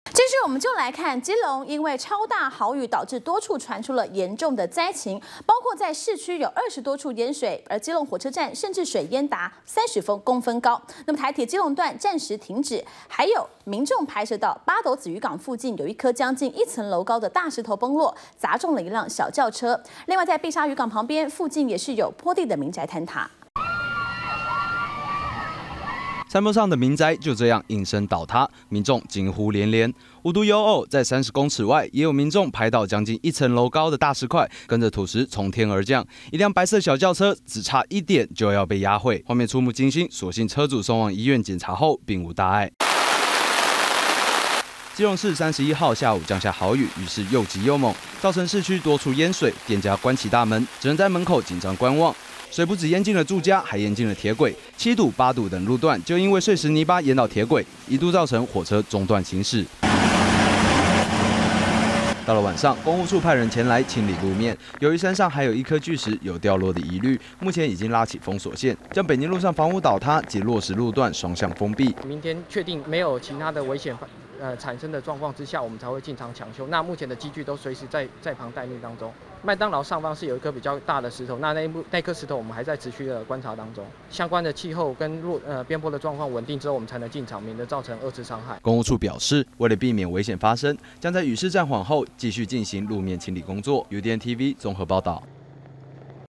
【2013.08.31】暴雨侵襲基隆 民宅崩塌巨石砸車 -udn tv
基隆因為大豪雨導致多處傳出嚴重災情，包括市區有20多處淹水，基隆火車站甚至水淹達30公分，台鐵基隆段暫時停駛。還有民眾拍攝到八斗子漁港附近一顆將近一層樓高的大石頭崩落，砸中一輛小轎車，在碧砂漁港附近也有坡地民宅坍塌。